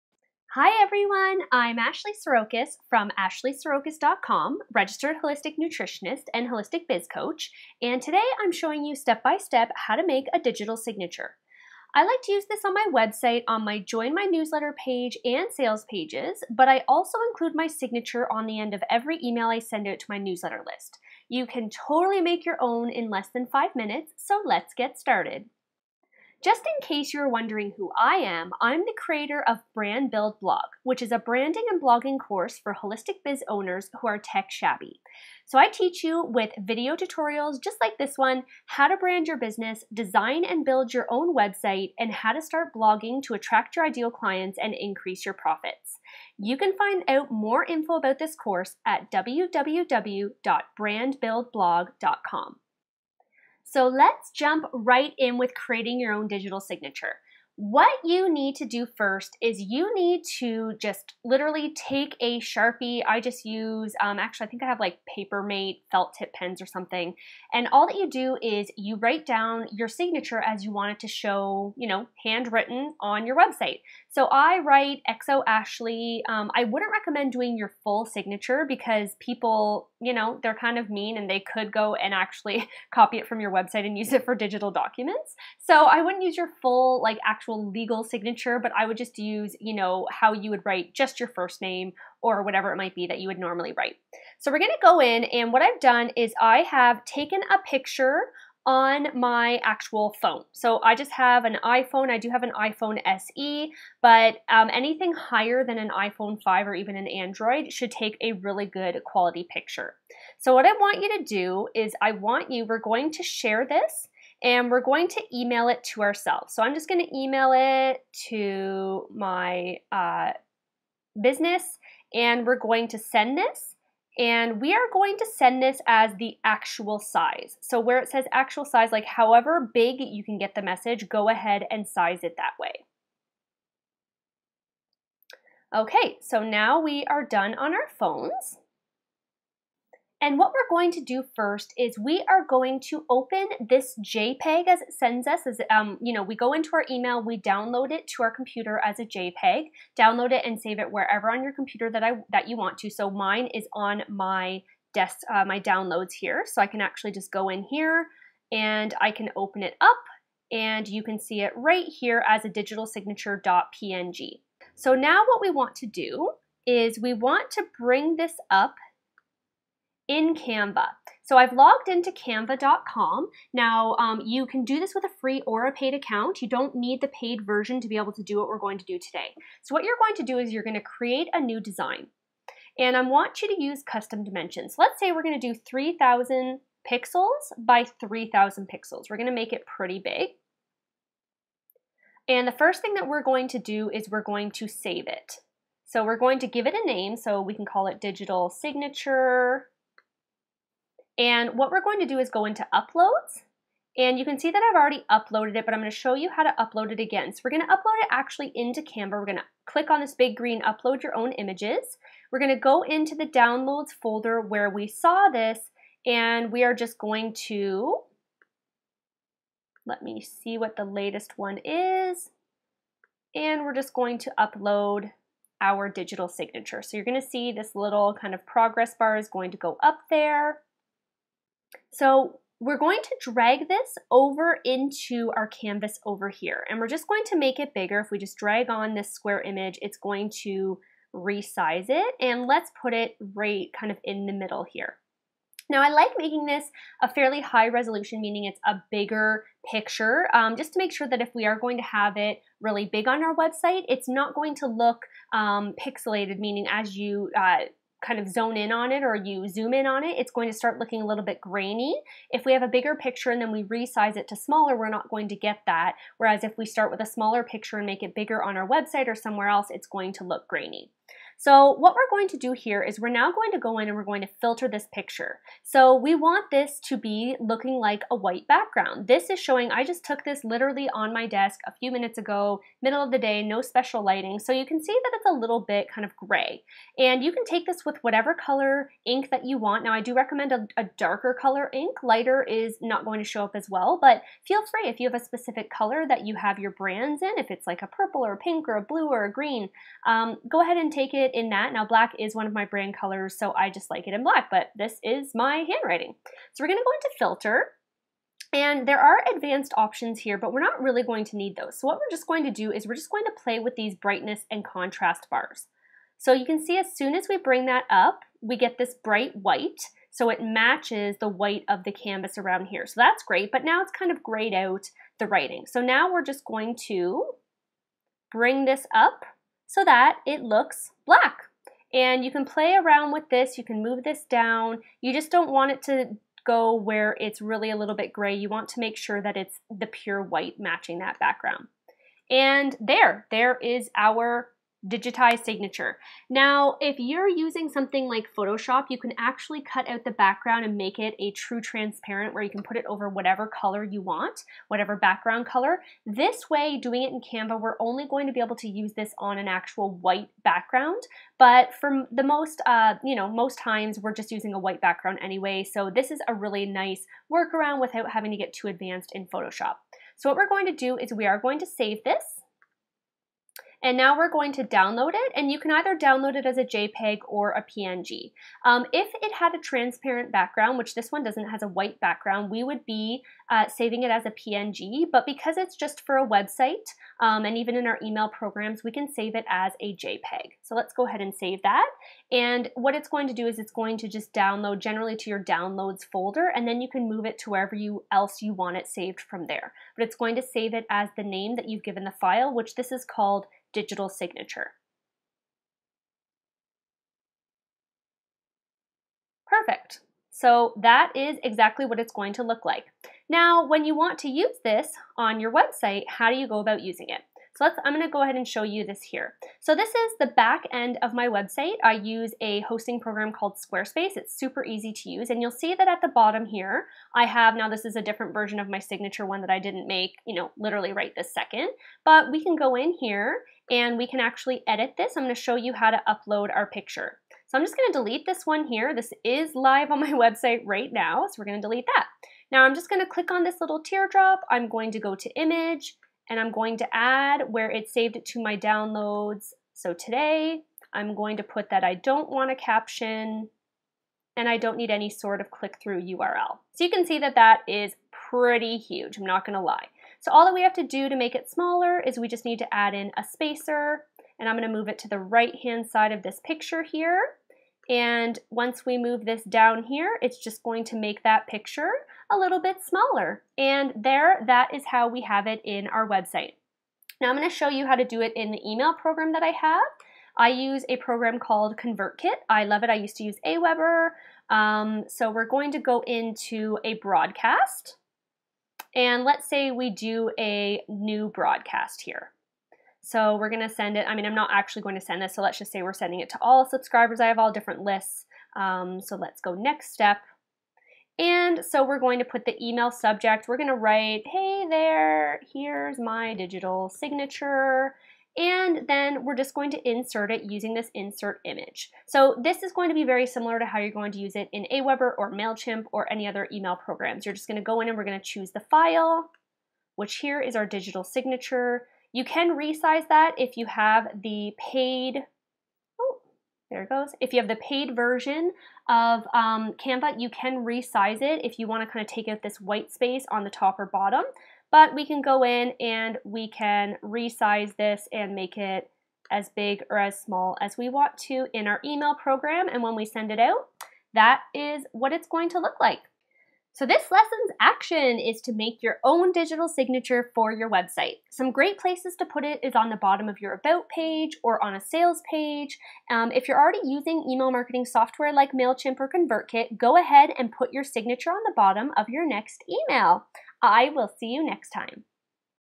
How to make a digital signature
Ashley Srokosz, Holistic Business Coach, shows you how to make a digital signature to use on your website and/or e-mail newsletter software in this step-by-step video tutorial.